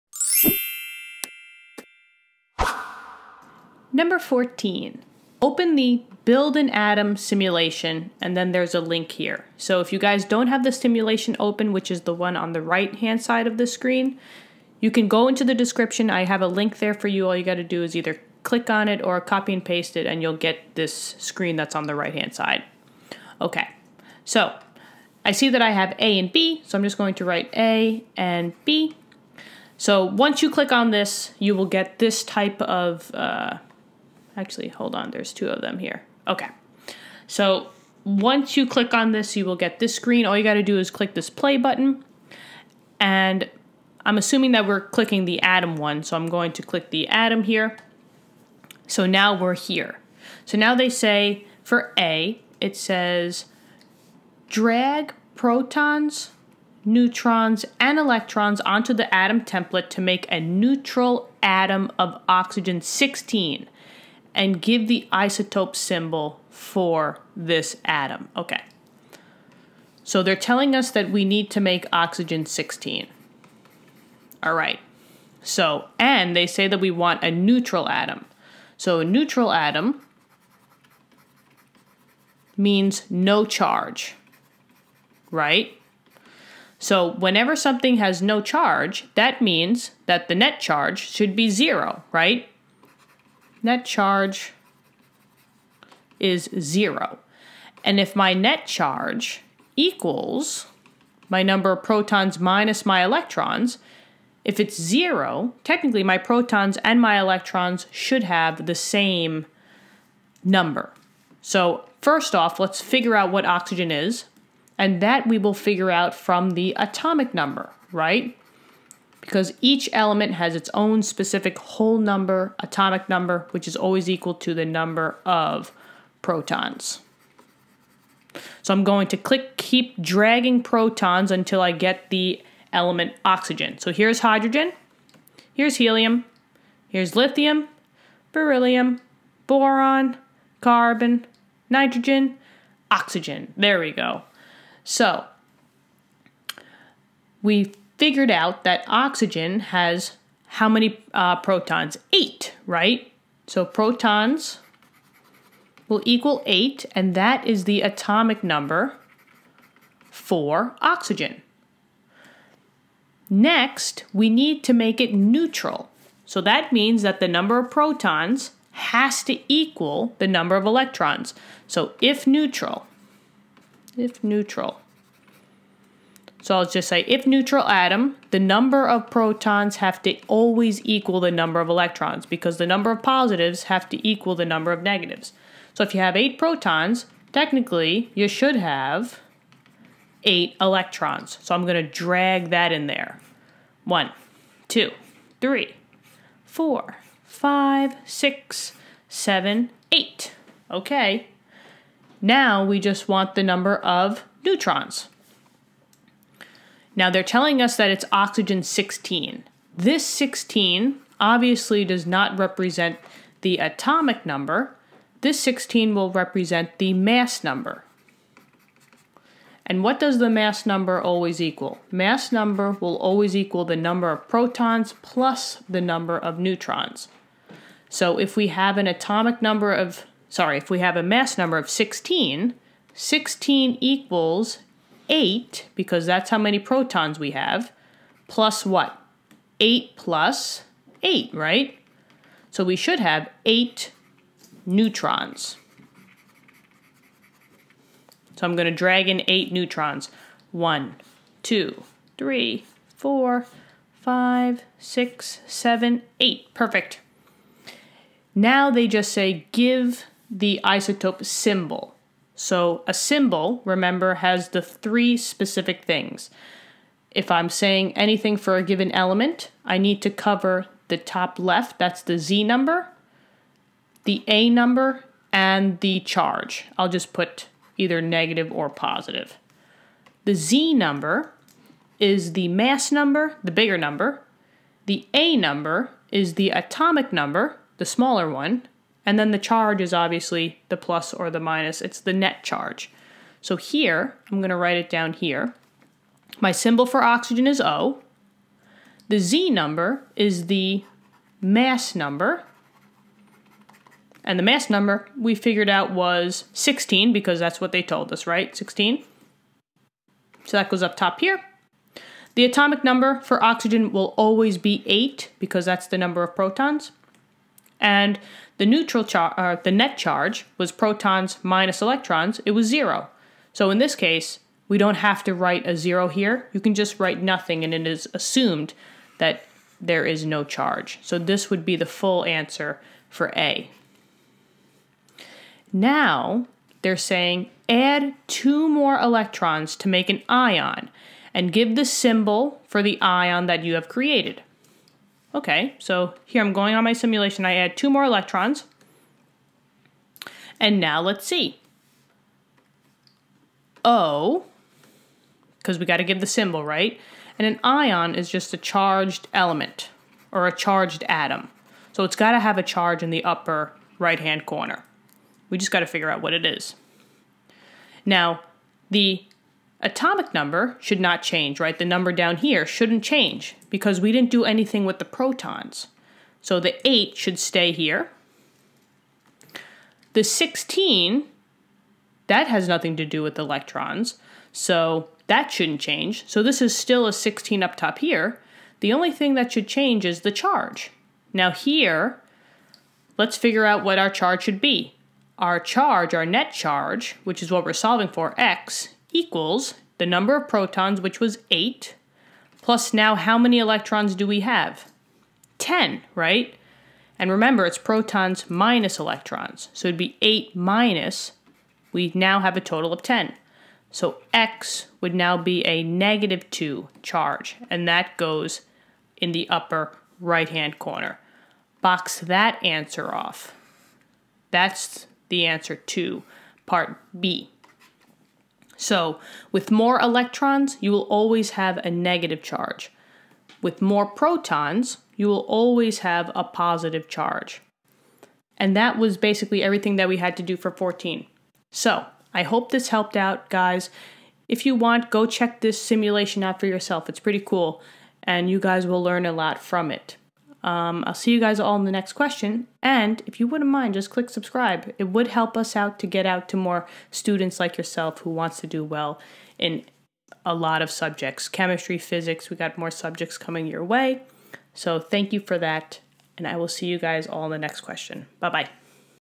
2.14 | Open the Build an Atom simulation ( (a) Drag protons
(a) Drag protons, neutrons, and electrons onto the atom template to make a neutral atom of Oxygen-16 and give the isotope symbol for this atom.
(b) Now add two more electrons to make an ion and give the symbol for the ion you have created.